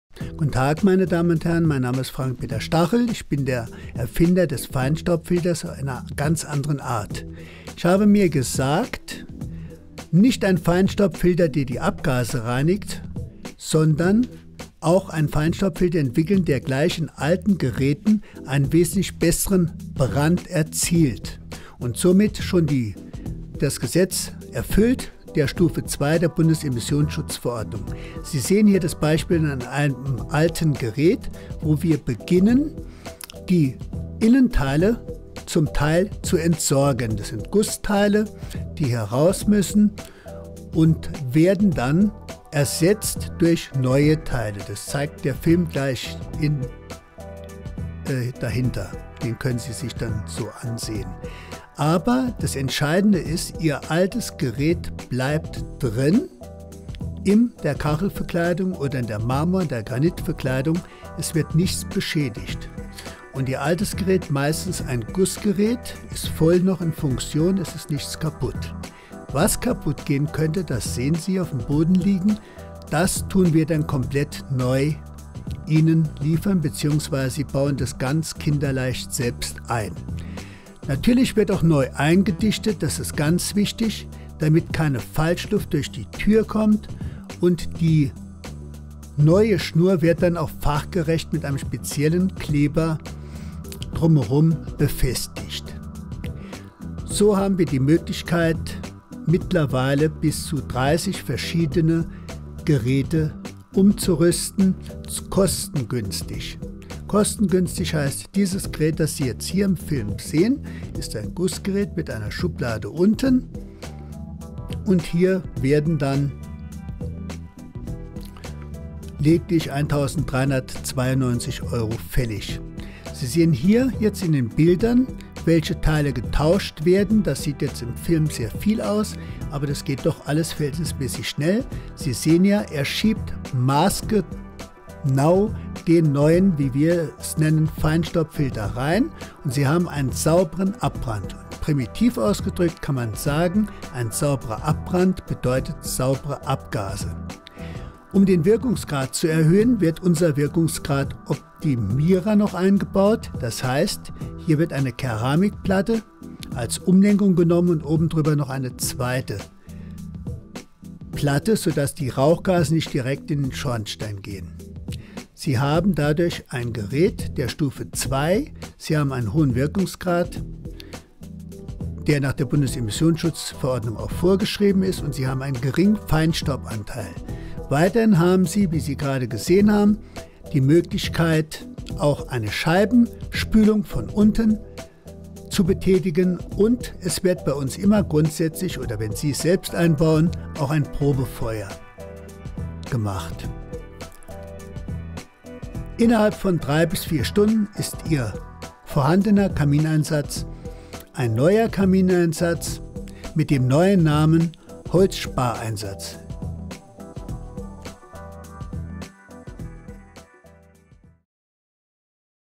STACHEL-FEINSTAUB-FILTER ®
Feinstaubfilter mit Holzbrand-Optimierung (HBO) für Kamineinsätze in denen Prüfung stattfand - stromlos und wartungsfrei
Alter Kamineinsatz plus Feinstaubfilter mit HBO ist Neugerät Stufe 2 nach BlmSchV.

Feinstaubfilter mit HBO: Unbegrenzte zeitliche Nutzung ohne Einschränkung. Passend für Kamineinsätze verschiedenster Hersteller. Sie erfüllen damit die Bundes-Immissionsschutz-Verordnung Stufe 2 für Neugeräte.